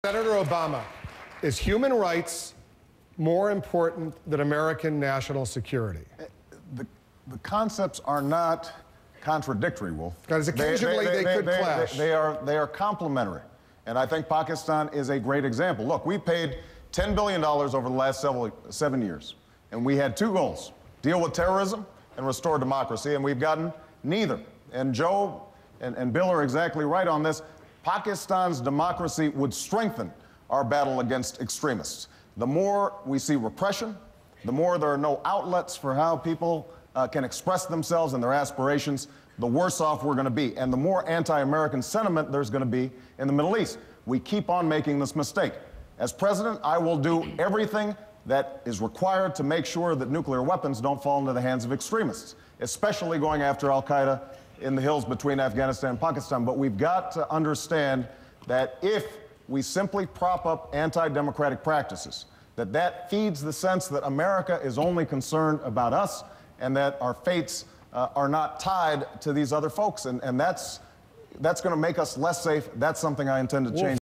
Las Vegas Debate: Barack Obama on Pakistan and human rights
Barack justifies a rational and forthright foreign policy that achieves security for Americans at the same time as standing up for human rights abroad